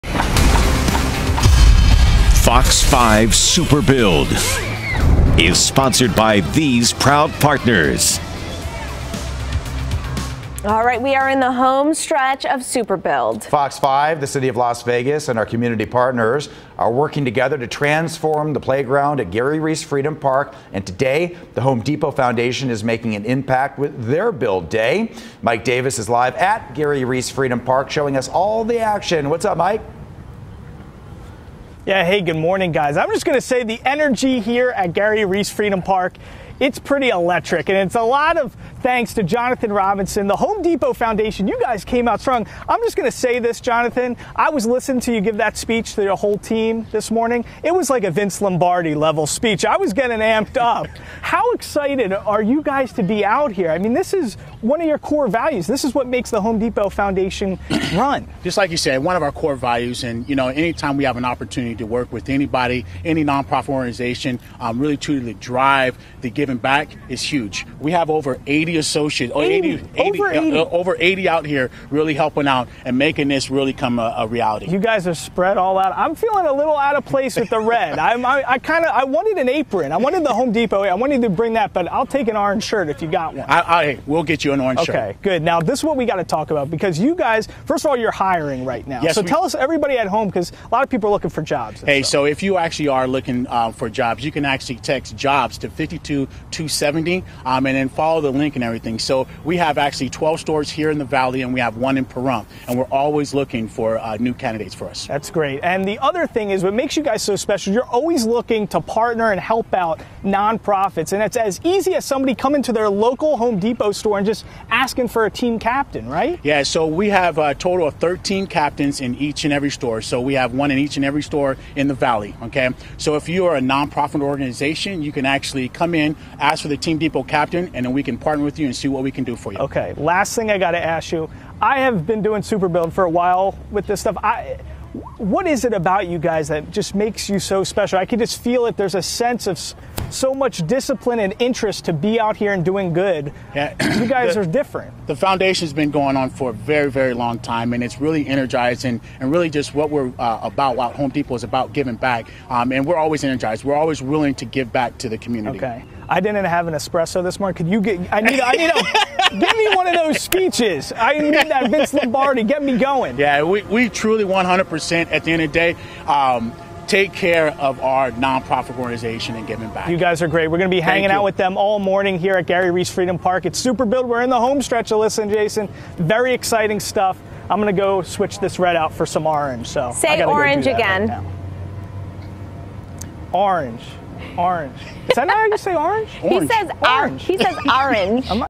Home Depot Team ready to hire and help!
More than sixty Home Depot teammates joined our Super Build, helping paint and cleanup trash.